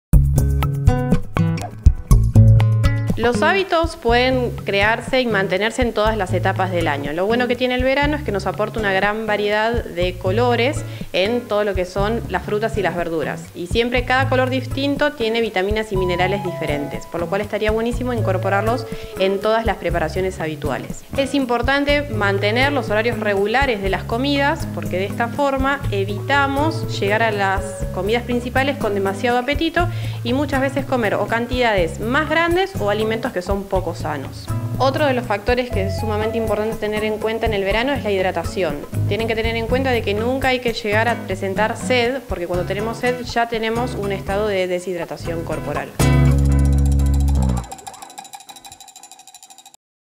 Verano: ¿Buen momento para una dieta saludable?
La Licenciada en Nutrición, Johanna López, nos cuenta la importancia de tener hábitos saludables en todo momento del año y aprovechar el verano para incorporar variedades de vitaminas.
“Los hábitos pueden crearse y mantenerse en todas las etapas del año. Lo bueno que tiene el verano es que nos aporta una gran variedad de colores en todas las frutas y las verduras. Cada color distinto tiene vitaminas y minerales diferentes, por eso estaría buenísimo incorporarlos en todas las preparaciones habituales”, explicó la profesional.
“Es importante mantener los horarios regulares de las comidas, porque de esta forma evitamos llegar a las comidas principales con demasiado apetito y comer cantidades más grandes o alimentos que son poco sanos”, remarcó.
Por último, hizo hincapié en que “otro de los factores importantes a tener en cuenta en el verano es la hidratación. Nunca hay que llegar a presentar sed porque cuando tenemos sed ya tenemos un estado de deshidratación corporal”.